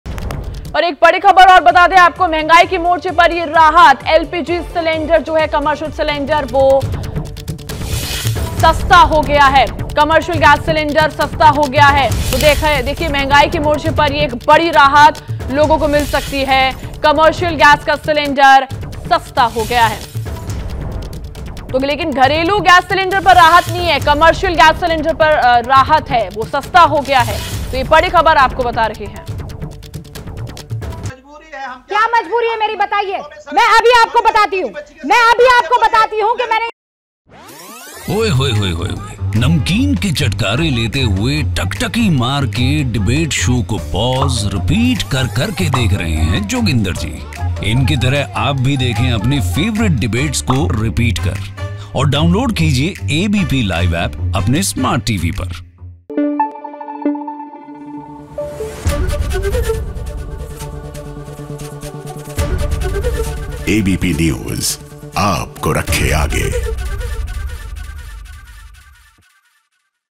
Commercial Cylinder Price: महंगाई से मिली बड़ी राहत, कमर्शियल सिलेंडर हुआ सस्ता
लंबे वक्त से महंगाई की मार झेल रहे आम लोगों को अब बड़ी राहत मिली है. महीने के पहले दिन यानी 1 अप्रैल 2023 को तेल विपणन कंपनियों ने अपने कस्टमर्स को बड़ी राहत देते हुए एलपीजी सिलेंडर के दामों के बड़ी कटौती कर दी है. कंपनियों ने 92 रुपये की कमी एलपीजी सिलेंडर के रेट्स में की है. ये कमी कमर्शियल सिलेंडर के रेट्स में की है. वहीं अगर घरेलू एलपीजी सिलेंडर की बात करें तो वह अपने पुराने रेट्स पर मिल रहे हैं. 92 रुपये की कटौती के बाद से ही दिल्ली समेत समेत पूरे देश में कमर्शियल सिलेंडर के दामों में कमी देखी गई है.